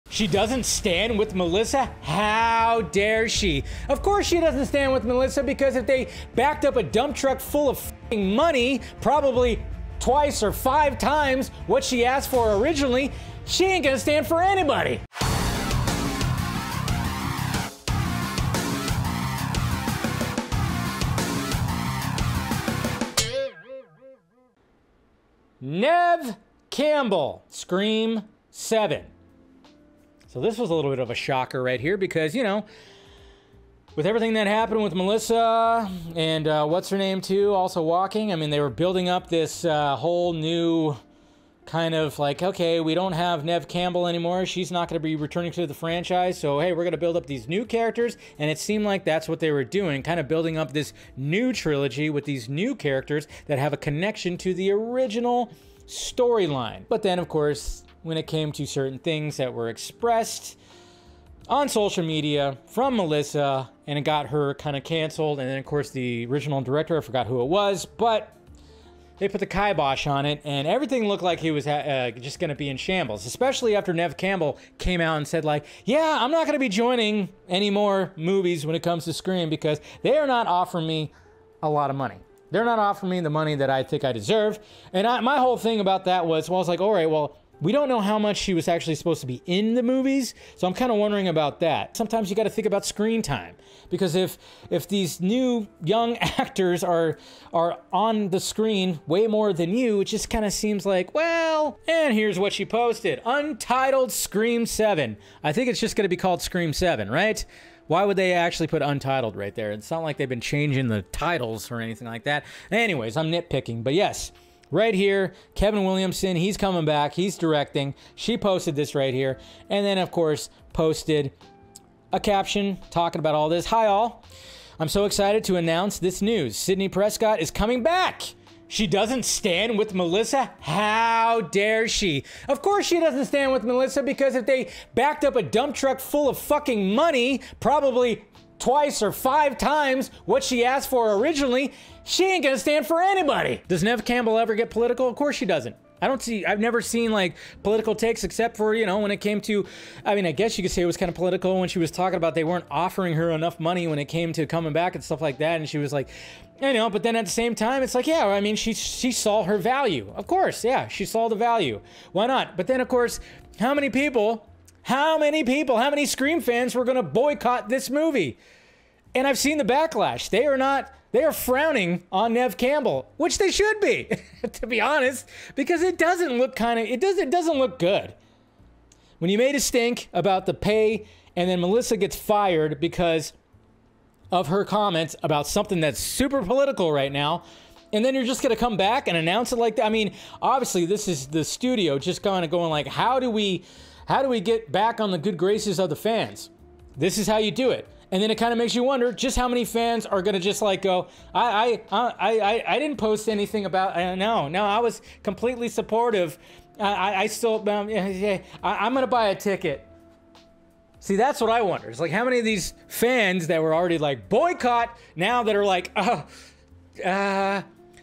Does Neve Campbell Scream 7 Announcement BETRAY Melissa Barrera?
Some Scream fans don't know how to feel about Scream 7 now that Neve Campbell is returning after the drama with Melissa Barrera.